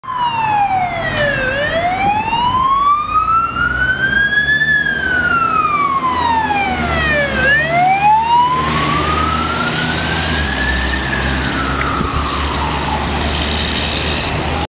Fire And Rescue NSW - Ladder Platform 18 Glebe
fire rescue new ladder platform 18 glebe responding to a building with smoke issuing from a apartment other units that responded Pumper 10 (redfern) and Pumper 18 (glebe) additional services responded from NSW ambulance and NSW police and energy australia